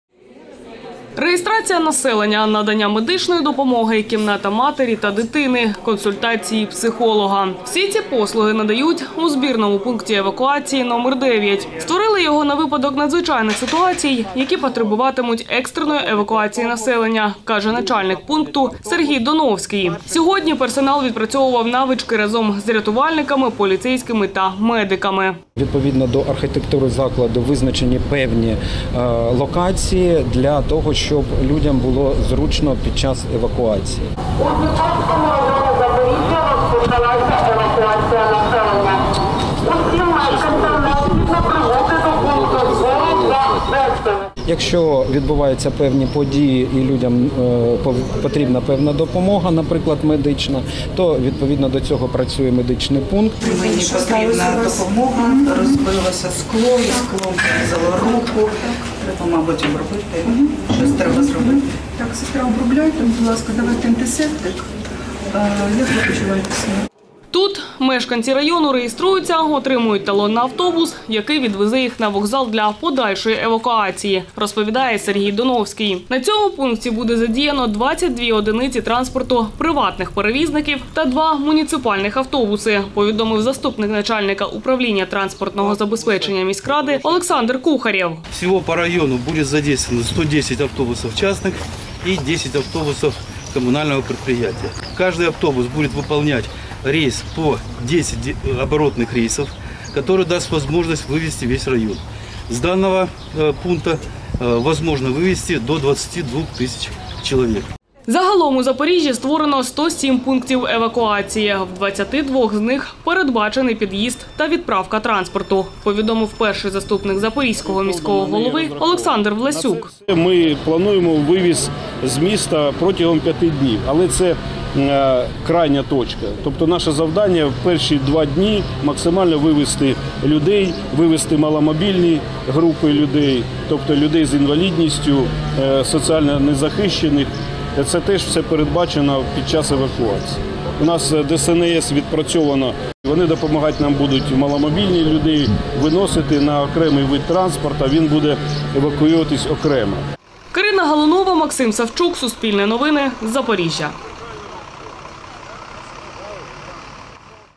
У Запоріжжі відпрацьовують план евакуації | Новини
Евакуювати населення за 5 днів. У Запоріжжі відпрацьовують дії у разі виникнення надзвичайних ситуацій. Для цього створили спеціальні пункти, в яких реєструватимуть людей, надаватимуть їм медичну допомогу, а потім евакуюватимуть автобусами. В одному з пунктів побували журналісти Суспільного.